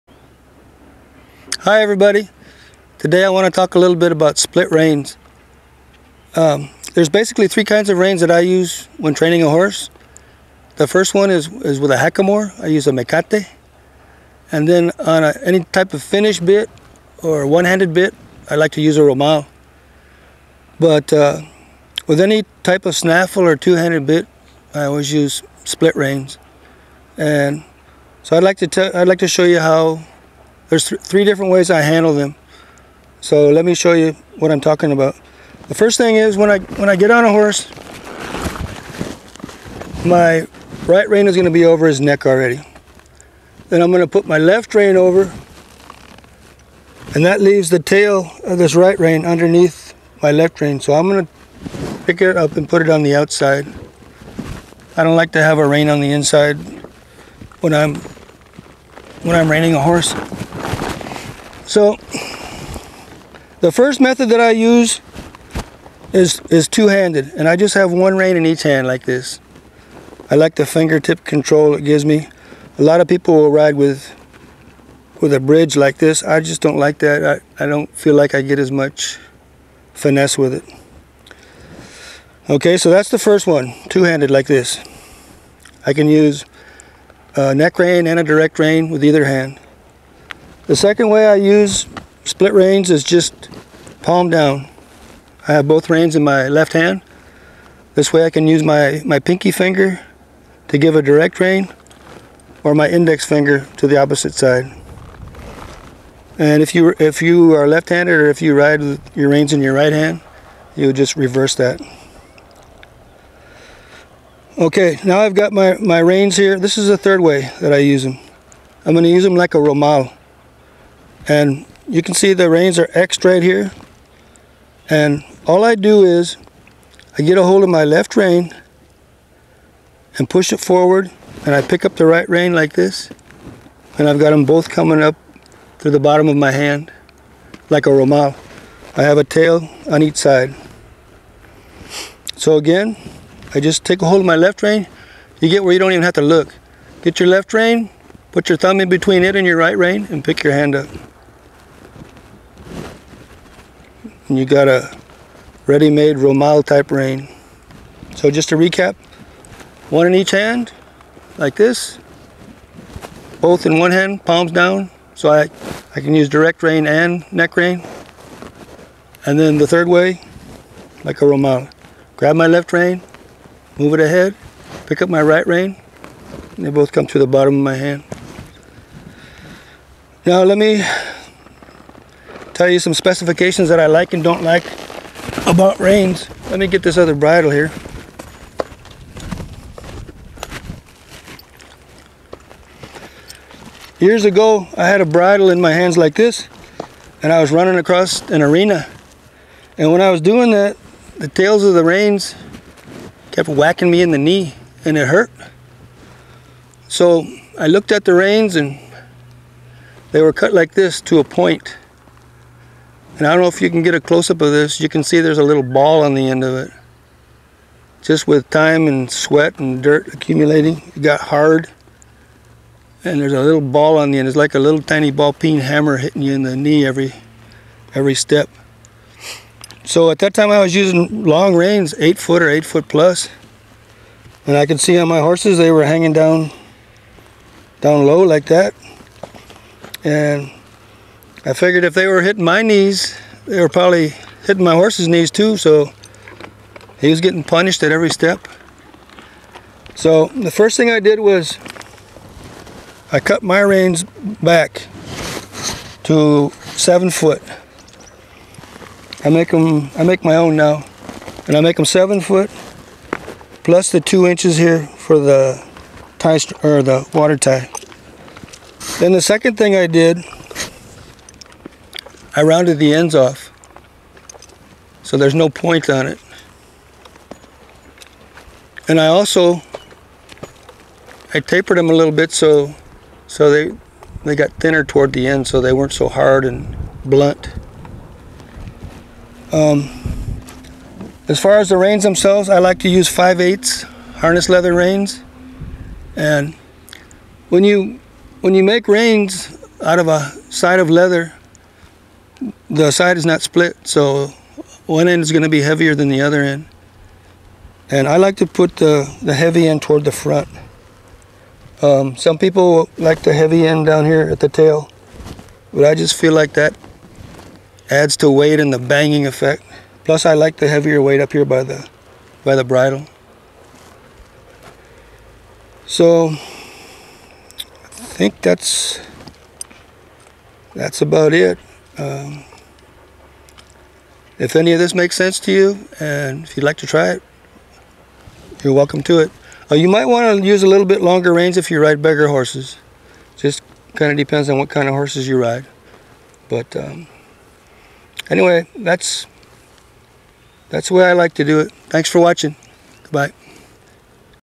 Tack Tip- Split Reins
Here we explain different ways to use split reins. I also talk about some of my personal preferences as far as their construction.